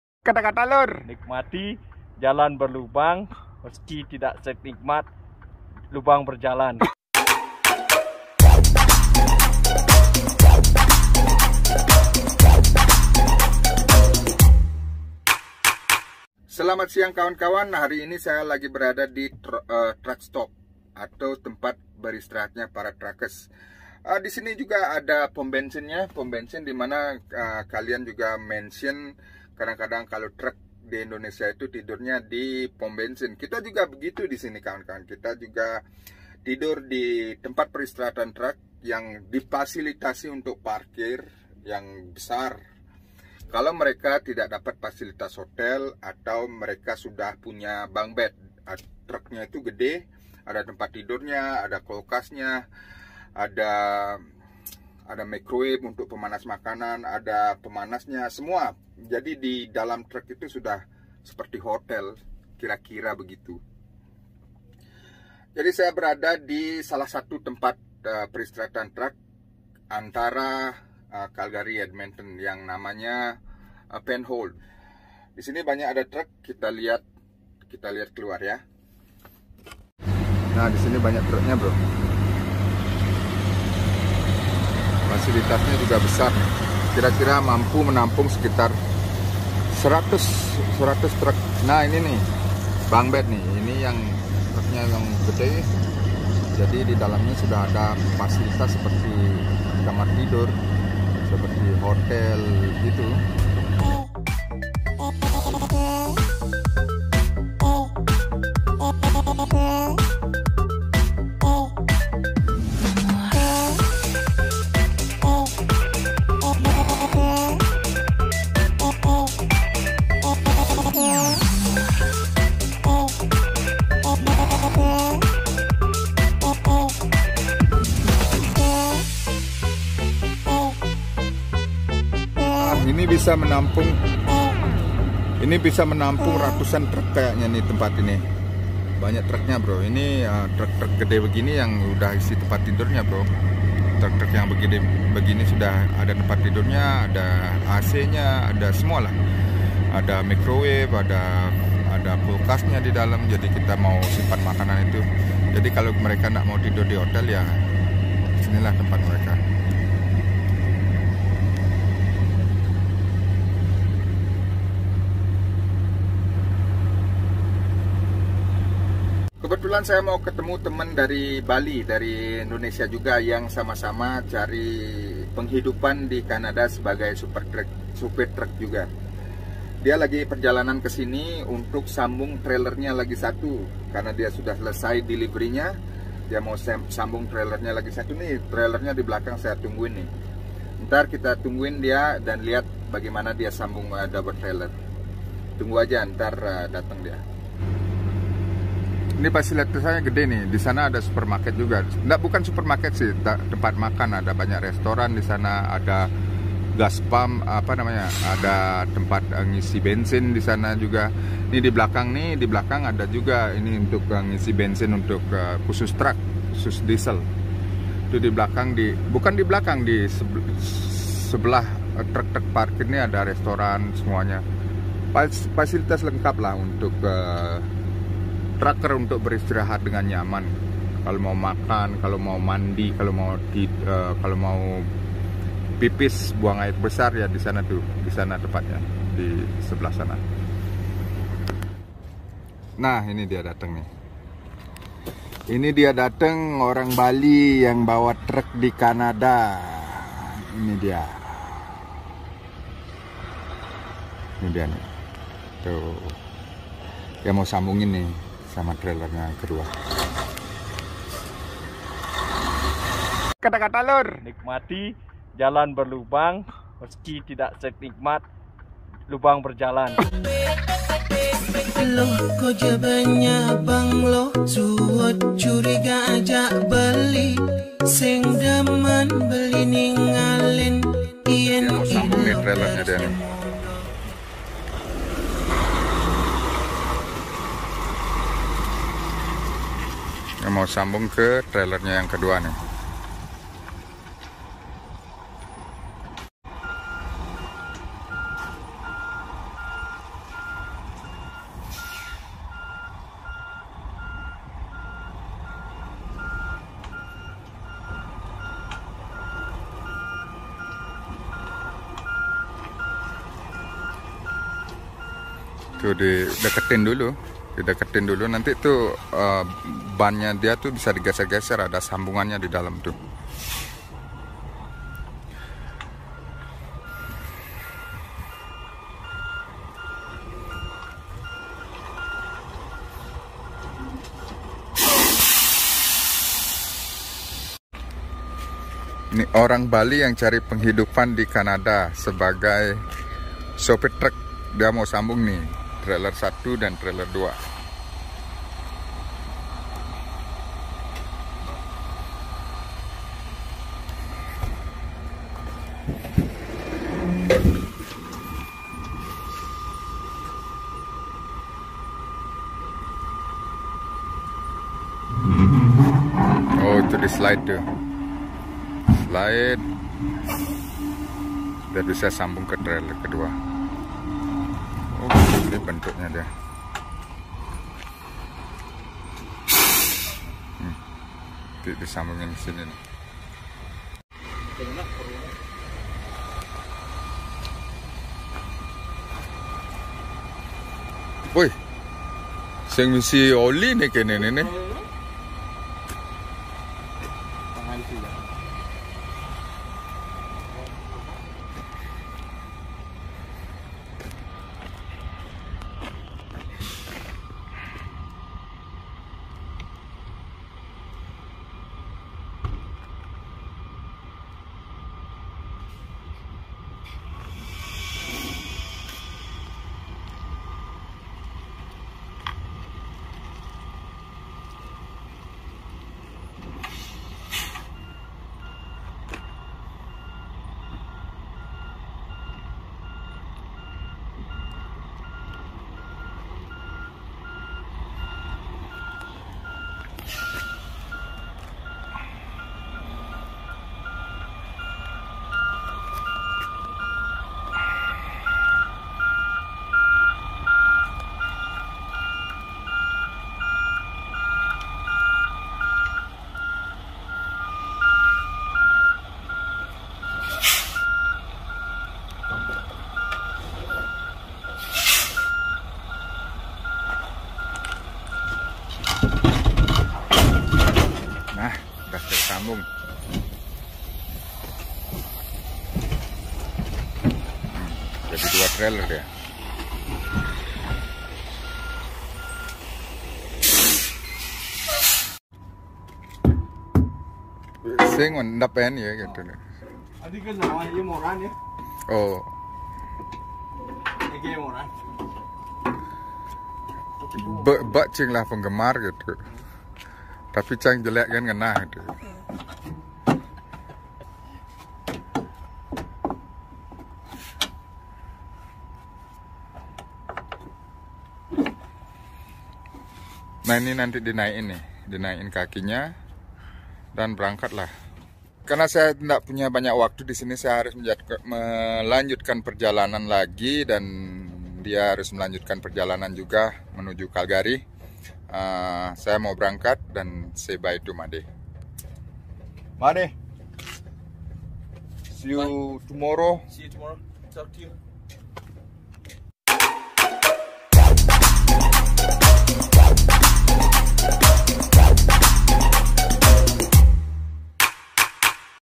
Supir Truk Panjang 25 Meter Ini Berasal Dari Bali Indonesia "Berpengalaman"
Supir truk panjang 25 meter ini dari bali Indonesia, berpengalaman dan bersahaja.
Supir truk panjang ini juga berjenggot, dan Garang. Kira Kira panjang truk yang dibawa mencapai 25 meter. kita lihat bagaimana supir truk panjang 25 meter ini sambung double trailer (sambung dua trailer.)